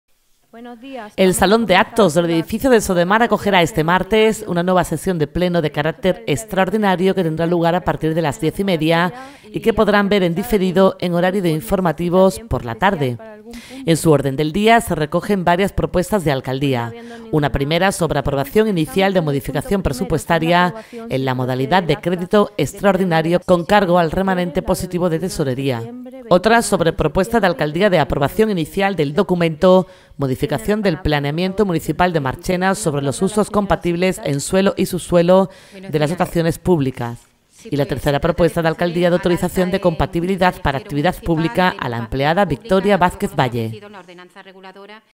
Este martes pleno extraordinario en el Ayuntamiento de Marchena
PUBLICA RTVMARCHENA

El salón de actos del edificio de Sodemar acogerá este martes una nueva sesión de pleno de carácter extraordinario que tendrá lugar a partir de las 10;30h y que podrán ver en diferido en horario de informativo por la tarde.

En su orden del día se recogen varias Propuestas de Alcaldía. Una primera sobre aprobación inicial de Modificación Presupuestaria en la modalidad de crédito extraordinario con cargo al remanente positivo de Tesorería.

Otra sobre Propuesta de Alcaldía de Aprobación Inicial del documento "Modificación del Planeamiento Municipal de Marchena sobre los usos compatibles en suelo y subsuelo de las dotaciones públicas".

Y la tercera Propuesta de Alcaldía de autorización de compatibilidad para actividad pública a la empleada Victoria Vázquez Valle.